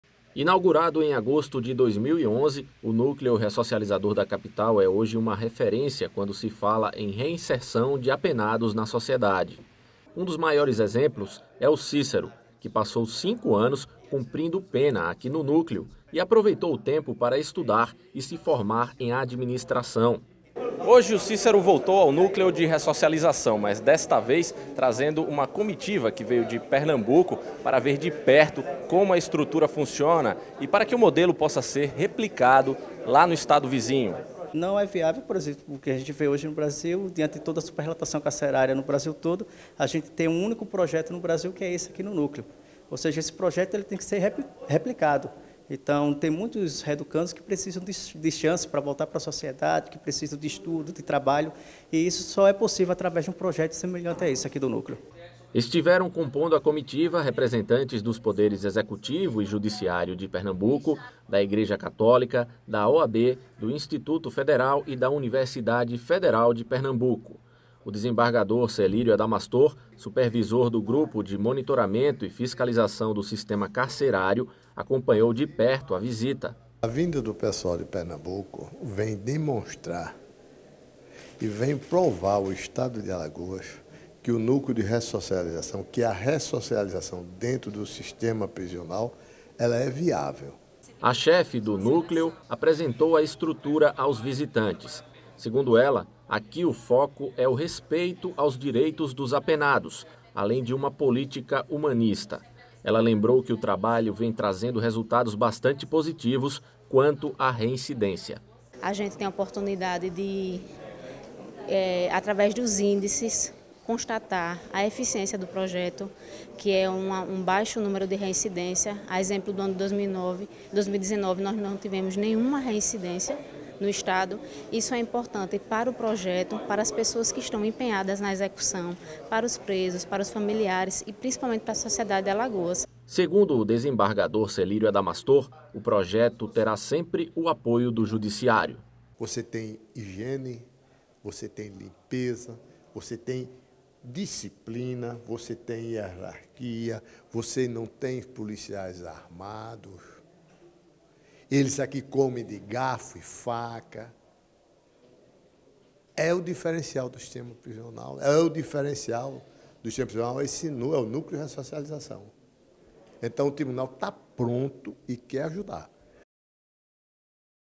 GMF recebe comitiva de PE no Núcleo Ressocializador
Pernambuco pretende construir um prédio para replicar a ideia bem-sucedida de Alagoas